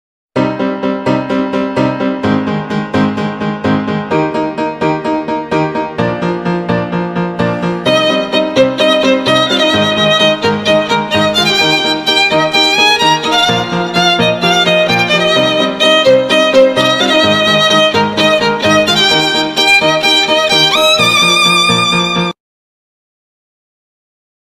Floor printer parking space color drawing machine
This is a floor painting machine, which can be used in all kinds of floor painting machines to save time and labor. Do you like this floor painting machine?